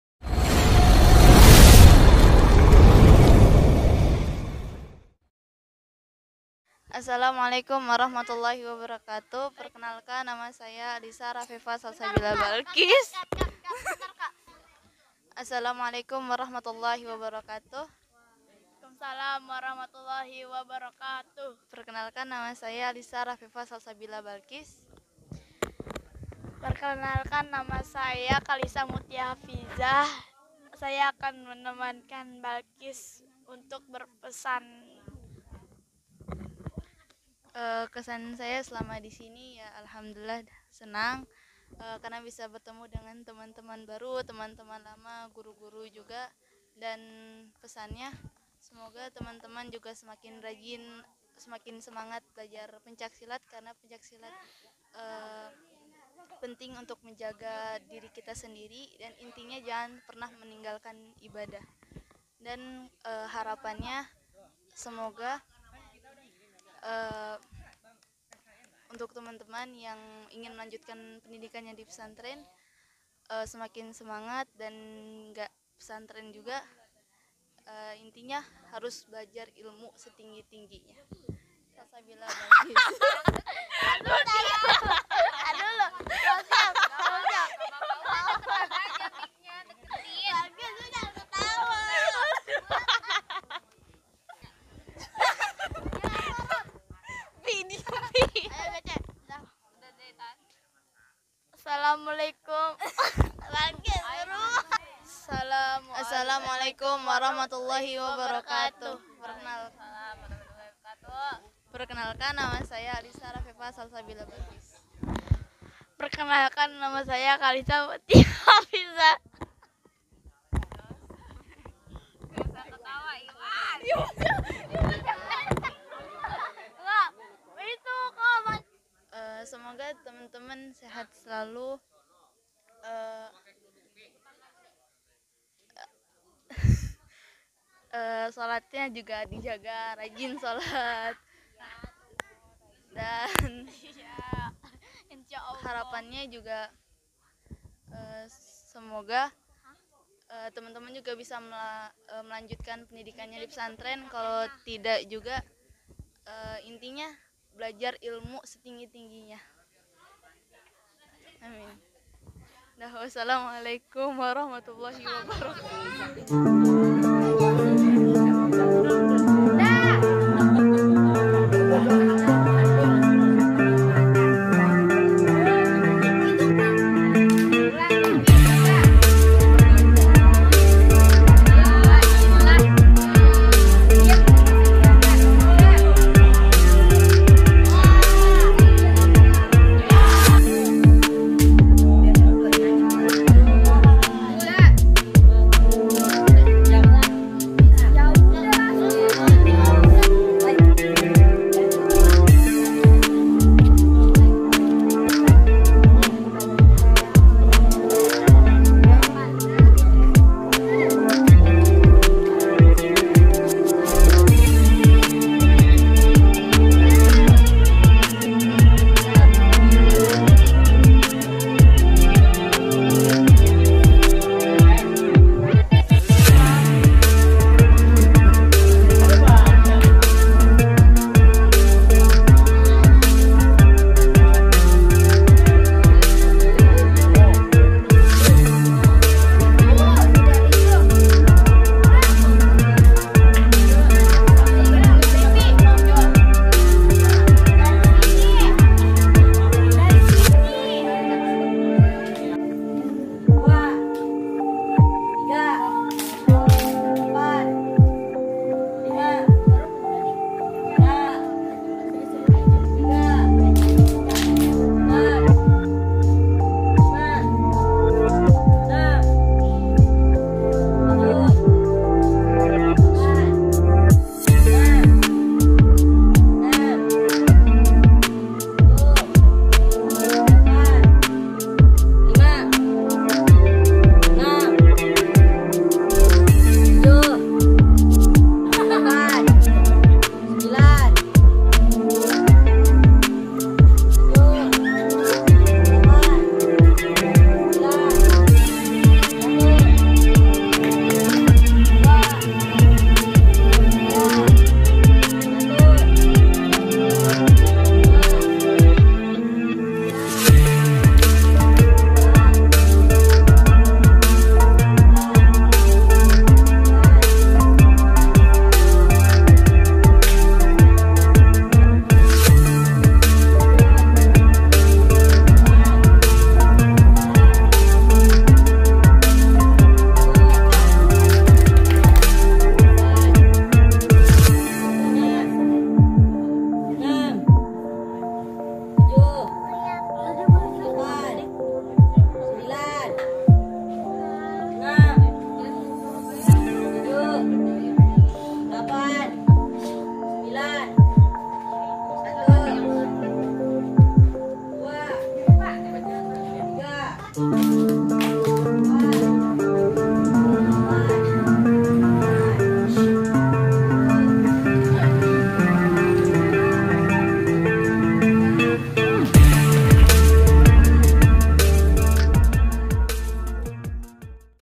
Perpisahan berangkat mondok 🔴 MS JALAN ENAM PENGASINAN Parakan Timur
Setiap tahunnya ada diantara kami yang melanjutkan pendidikan ke jenjang Pondok Pesantren, semoga diberikan kemudahan dalam menuntut ilmu, diberikan kesabaran, diberikan kekuatan, dan kesehatan.
Ada 6 anak yang berangkat di tahun ini, berkah insha Allah.